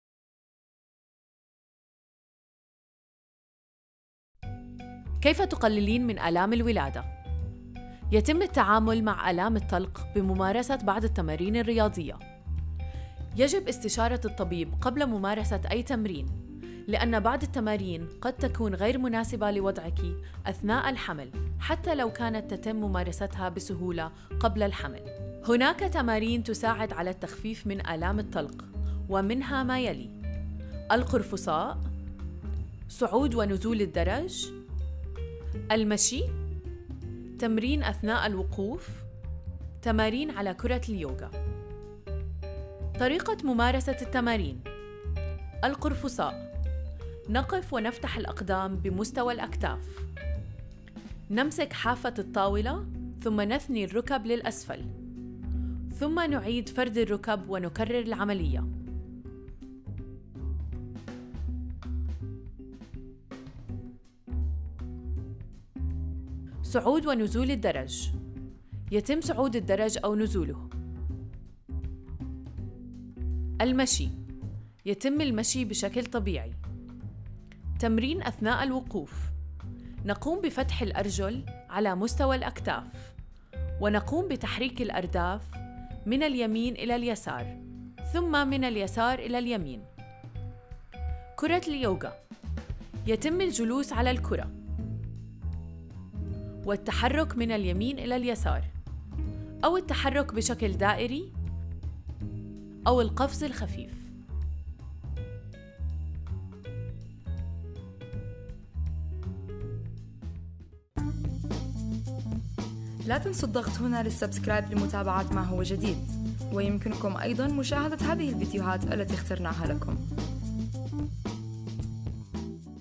كيف تقللين من الام الولادة مع رولى؟
كيف تقللين من الام الولادة مع رولا

الخطوات:
 يتم التعامل مع الآم الطلق بممارسة بعض التمارين الرياضية.

يجب اسشتارة الطبيب قبل ممارسة أي تمرين. لأن بعض التمارين قد تكون غير مناسبة لوضعك أثناء الحمل حتى لو كانت تتم ممارستها بسهولة قبل الحمل.

هناك تمارين تساعد على التخفيف من الآم الطلق و منها ما يلي:
1.       القرفصاء
2.       صعود و نزول الدرج
3.       المشي
4.       تمرين أثناء الوقوف
5.       تمارين على كرة اليوغا.

طريقة ممارسة التمارين:
1. القرفصاء:
نقف و نفتح الأقدام بمستوى الأكتاف, نمسك بحافة الطاولة ثم نثني الركب للأسفل و ثم نعيد فرد الركب و نكرر العملية.
2. صعود و نزول الدرج.
يتم صعود و نزول الدرج بشكل طبيعي.
3. المشي.
يتم المشي بشكل طبيعي.
4.تمرين أثناء الوقوف:
نقوم بفتح الأرجل على مستوى الأكتاف و نقوم بتحريك الأرداف من اليمين إلى اليسار ثم من اليسار إلى اليمين.
5. كرة اليوغا:
يتم الجلوس على الكرة و التحرّك من اليمين إلى اليسار أو التحرك بشكل دائري أو القفز الخفيف.